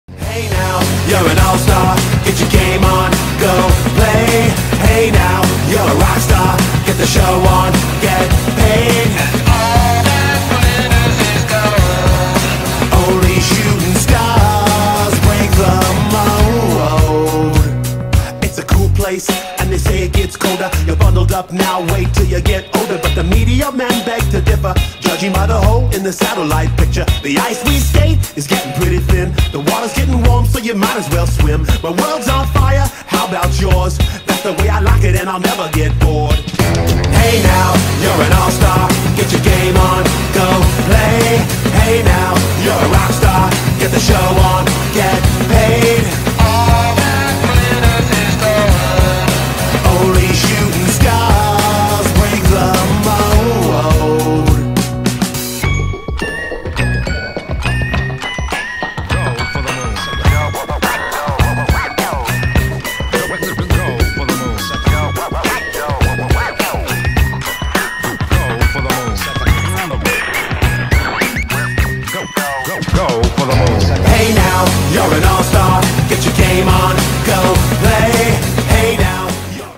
John Scott, the NHL All Star
Let's free John Scott, the true All Star. If you like this, please like it and share it on Twitter or Facebook with the hastag freejohnscott and maybe, one day, we can save this man from the NHL.